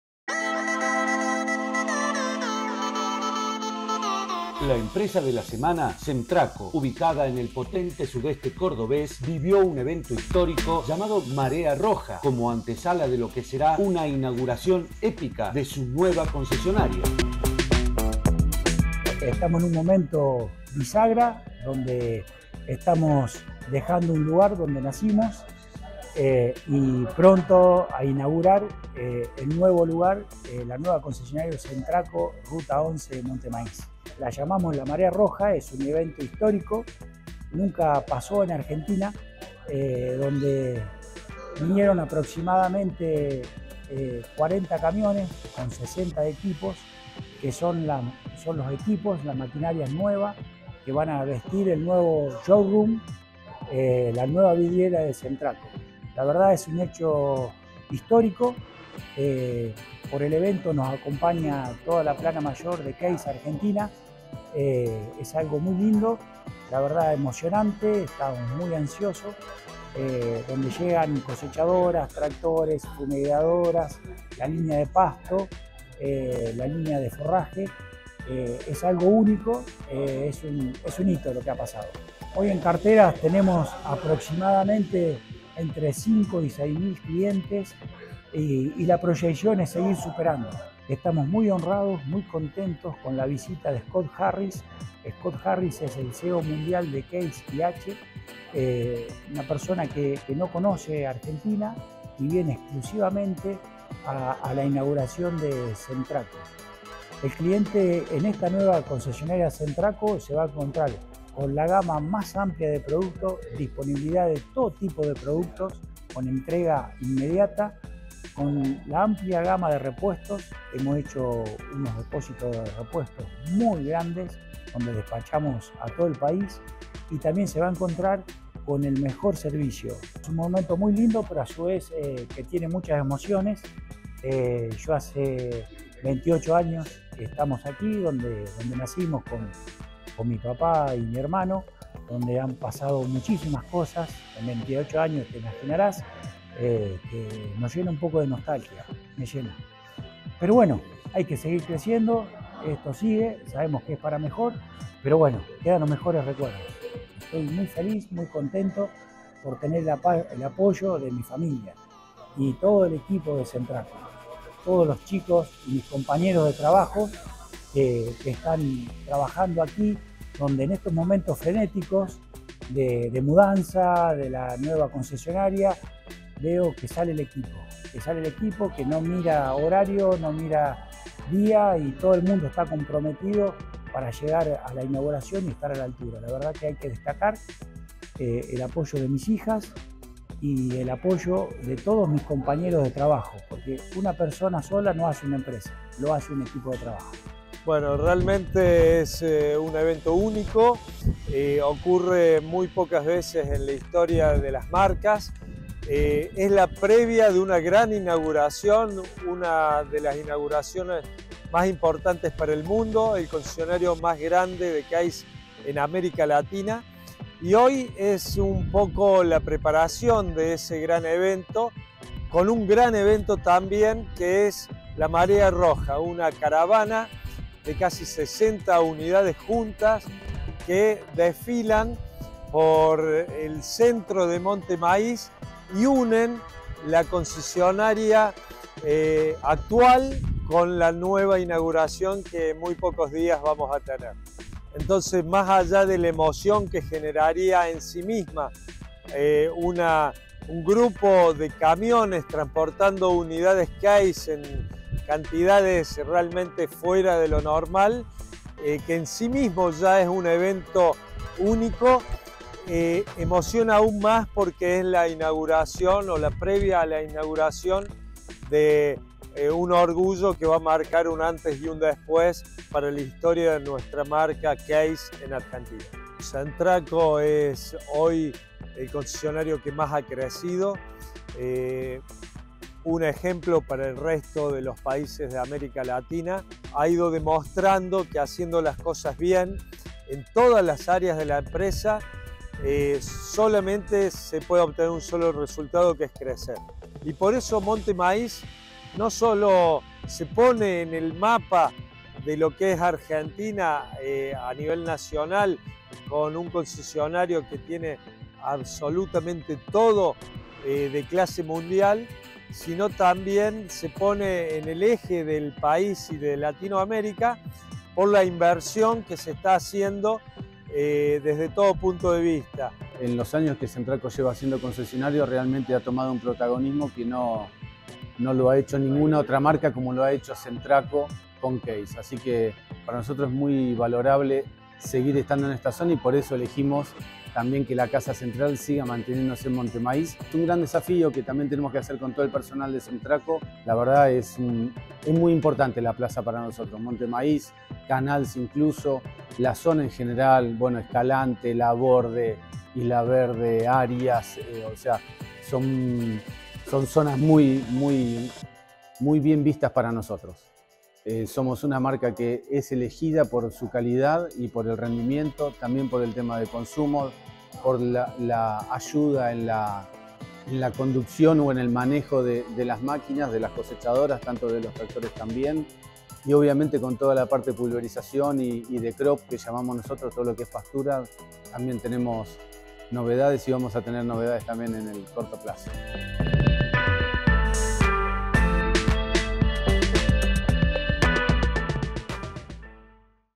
Semtraco estrena su nuevo concesionario con una inauguración épica
Semtraco vivió una histórica “Marea Roja” como antesala de la inauguración de su concesionaria en el sudeste de Córdoba, donde más de 35 camiones con 60 maquinarias arribaron a la nueva sede. Javier Nebbia, CEO de la empresa, nos habla sobre la dimensión de este suceso.

Encontrá las noticias más relevantes del mundo del comercio exterior y las empresas en nuestra web